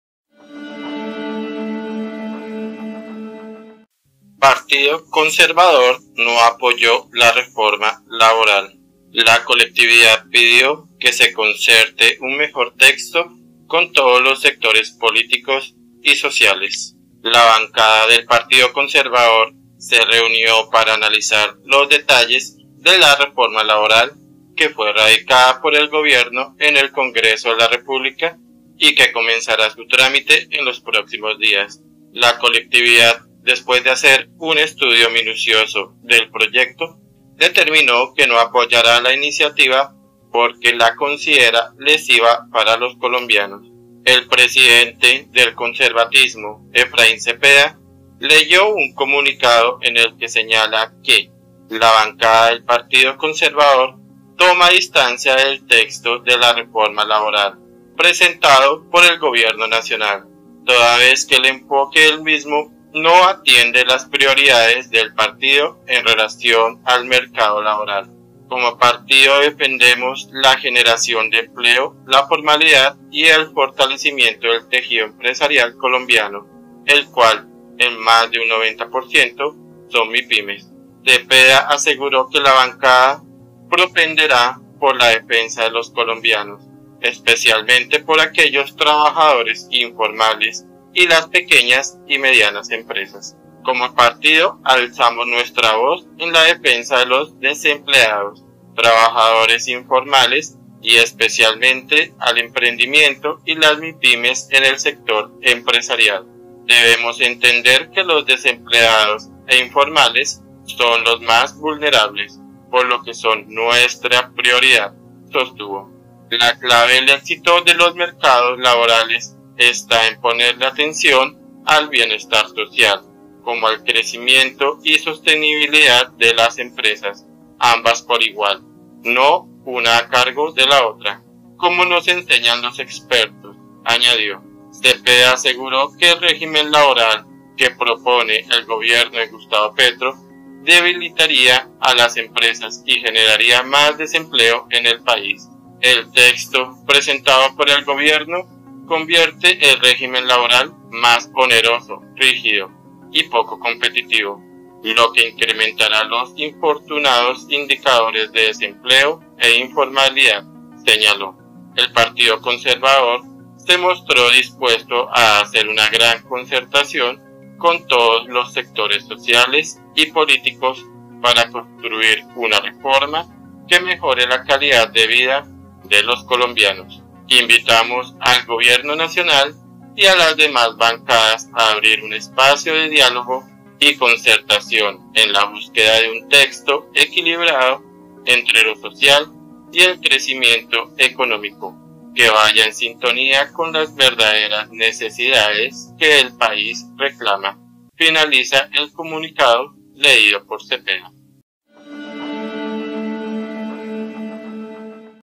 MÁS PROBLEMAS PARA EL GOBIERNO PETRO ⛔ CONSERVADORES CONTRA LA LABORAL
Partido Conservador no apoyó la reforma laboral
La colectividad pidió que se concerte un mejor texto con todos los sectores políticos y sociales.